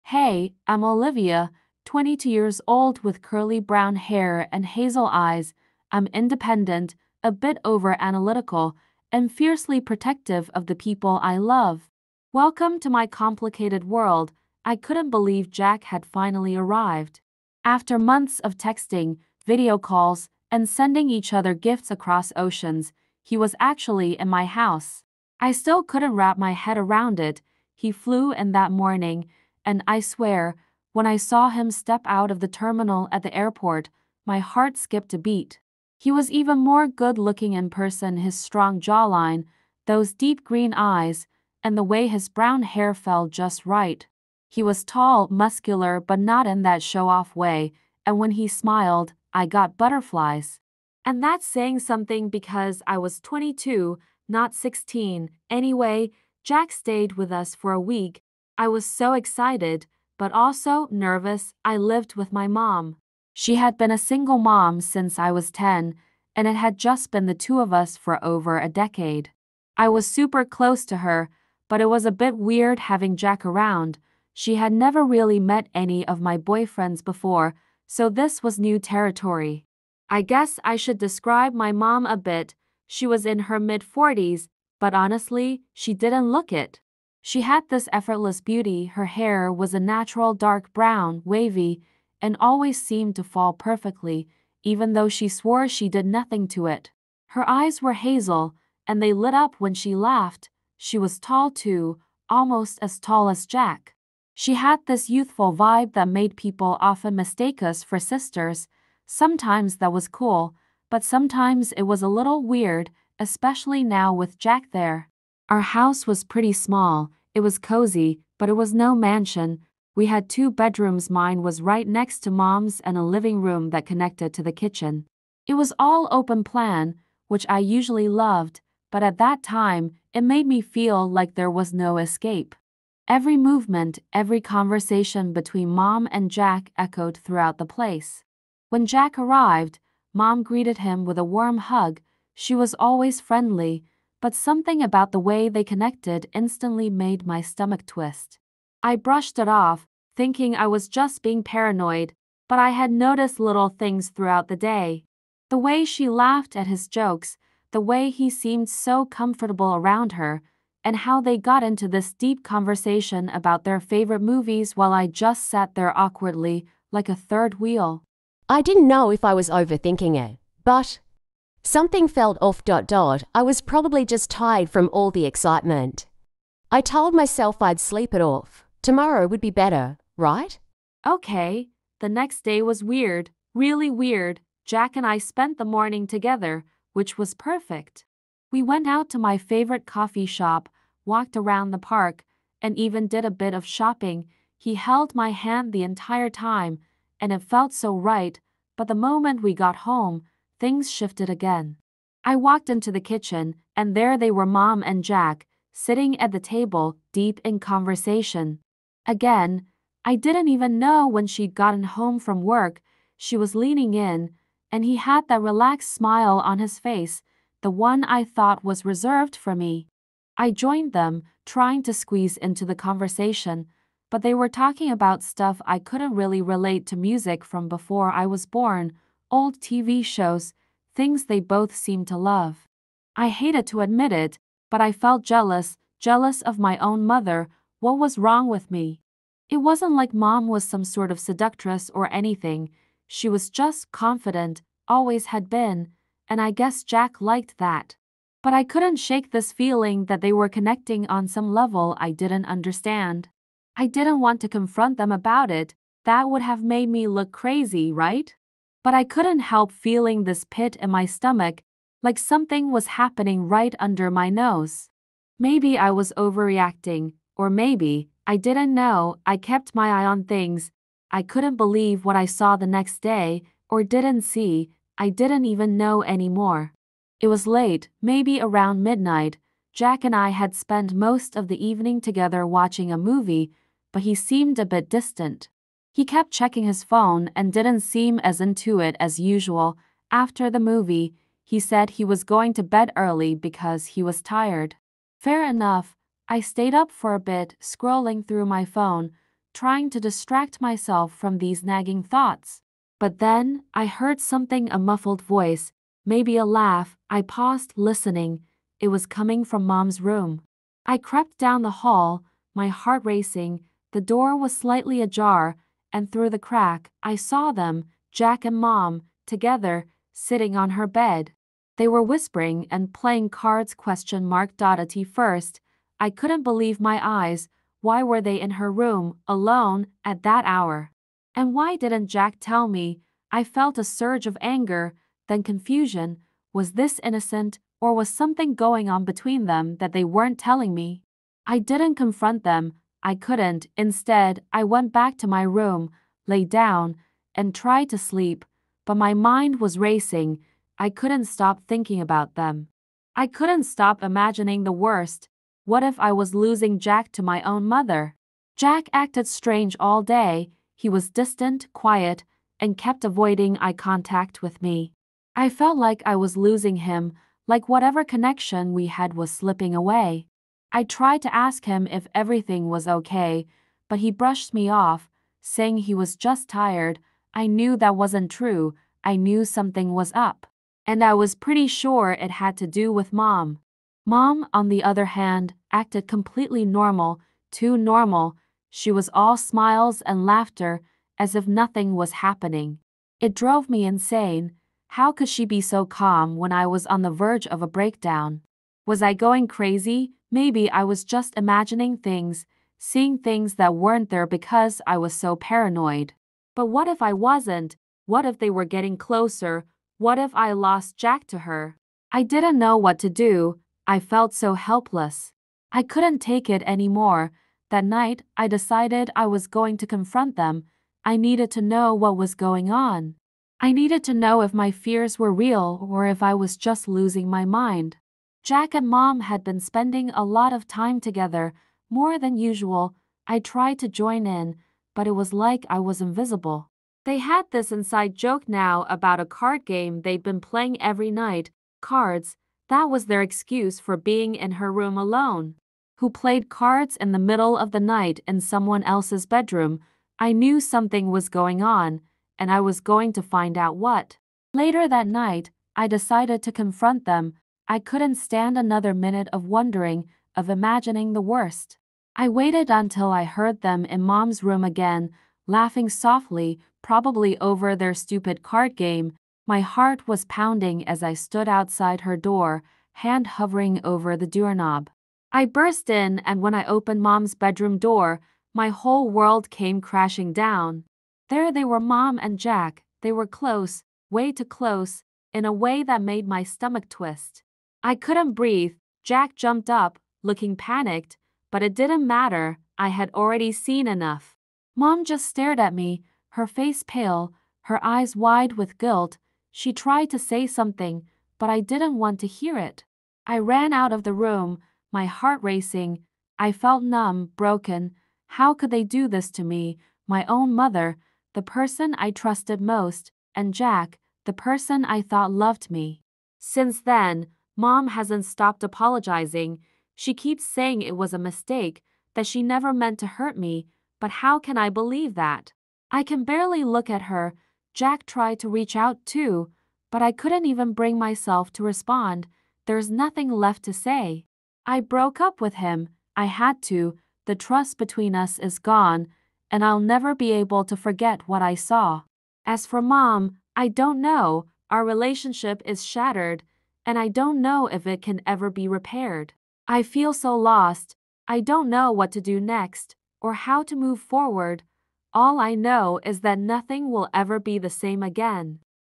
My Mother SEDUCED My Boyfriend While I Was Sleeping At Night | A true infidelity story
Prepare for a shocking tale of betrayal as I uncover the ultimate act of infidelity between my mother and my boyfriend. In this jaw-dropping story, you'll witness how a once loving relationship turned into a scandalous affair that shattered my trust and family. If you're into stories about cheating, toxic relationships, family drama, or love triangles, this video is for you. Watch as I reveal every heart-wrenching detail and how I confronted the ultimate betrayal. Don't miss this emotional rollercoaster that will leave you questioning loyalty and love.